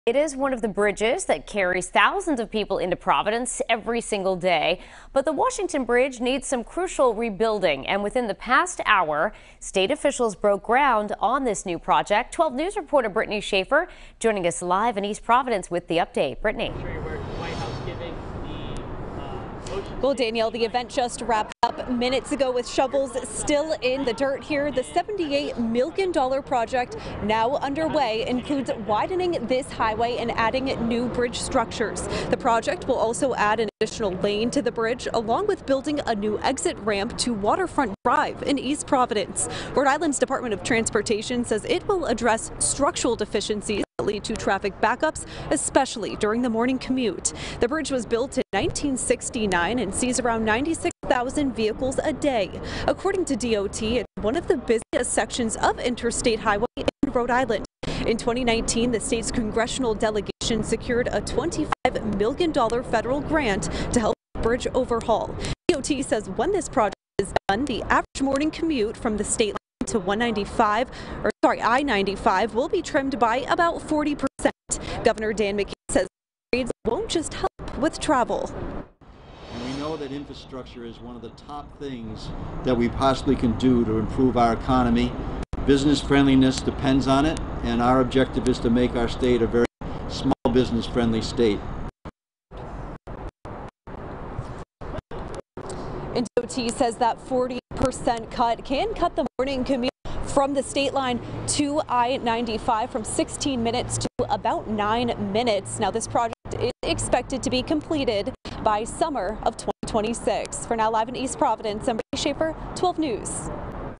State leaders break ground on $78M Washington Bridge project
The Washington Bridge carries thousands of people into Providence every day but needs some crucial rebuilding.